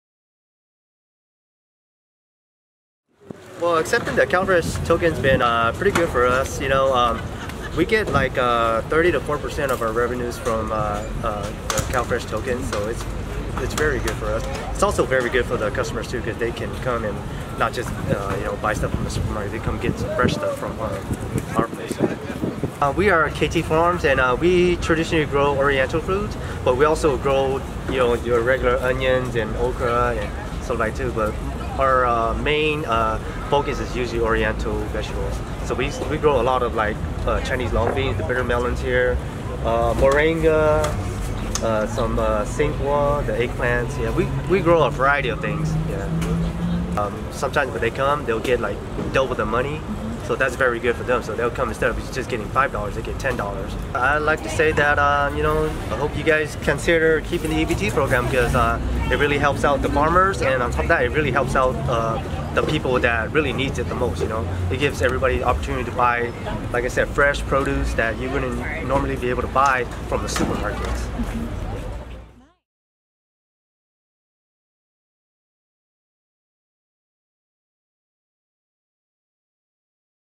Voices of SNAP: Yia
Yia is a second generation farmer who relies on CalFresh to support his business.